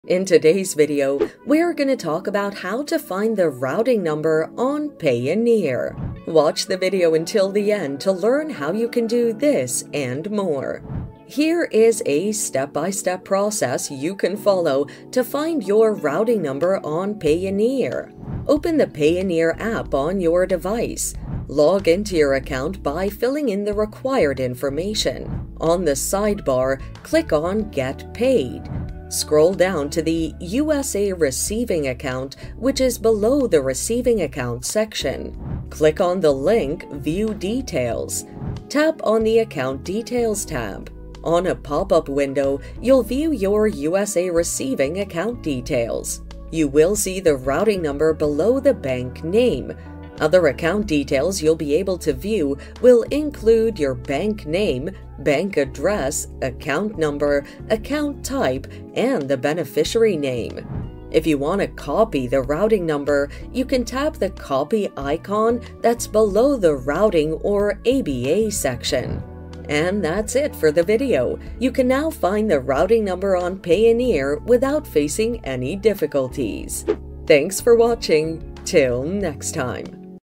How To Find The Routing Number Payoneer (How To Get Payoneer Account Details)
How To Find The Routing Number Payoneer (How To Get Payoneer Account Details). In this video tutorial I will show how to find the Routing Number Payoneer.

Here is a step-by-step process you can follow to find your routing number on Payoneer.
1. Open the Payoneer app on your device.
2. Login to your account by filling in the required information.
3. On the sidebar, click on “Get paid”.
4. Scroll down to the USA Receiving account which is below the Receiving accounts section.
5. Click on the link “View details”.
6. Tap on the Account details tab.
7. On a pop-up window, you will view your USA-receiving account details.
8. You will see the routing number below the bank name. Other account details, you will be able to view will include; bank name, bank address, account number, account type, and the beneficiary name.
If you want to copy the routing number you can tap the copy icon that is below the Routing (ABA) section.